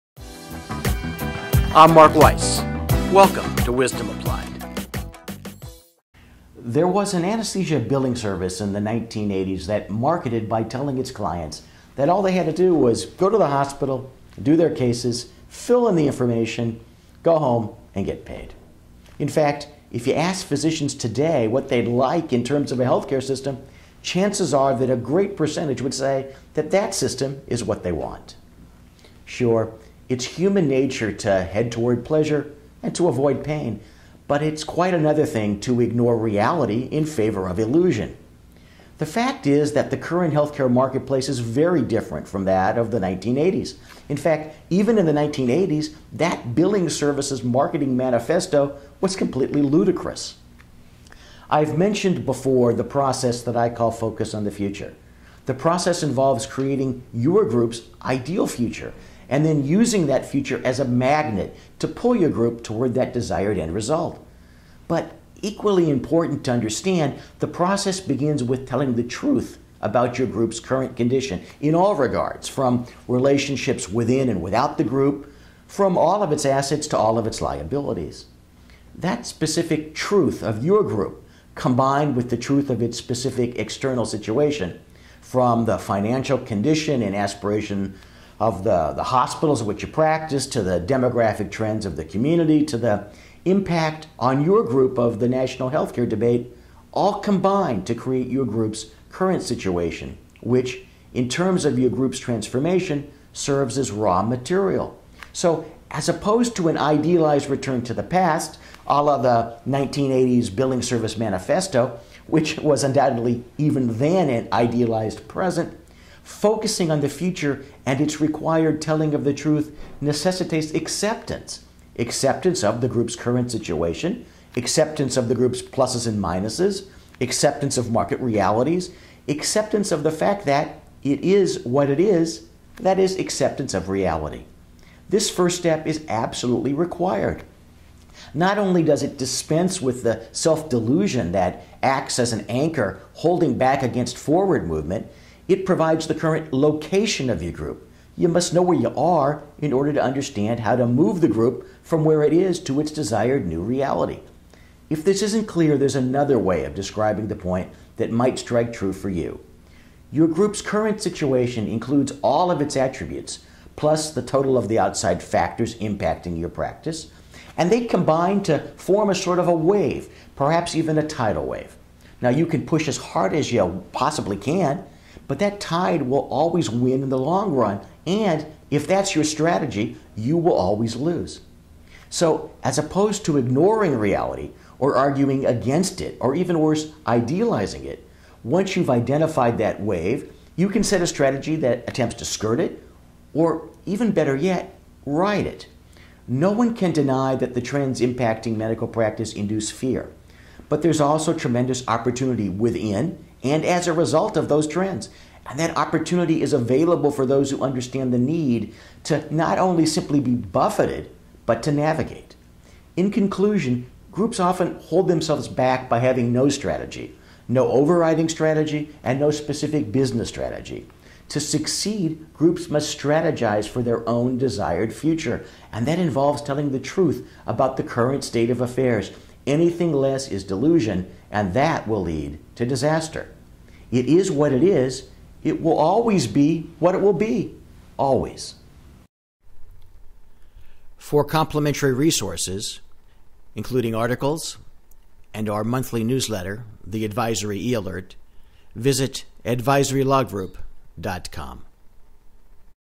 Wisdom Applied 53 - It Is What It Is
To succeed, groups must strategize for their own desired future and that involves telling the truth about the current state of affairs.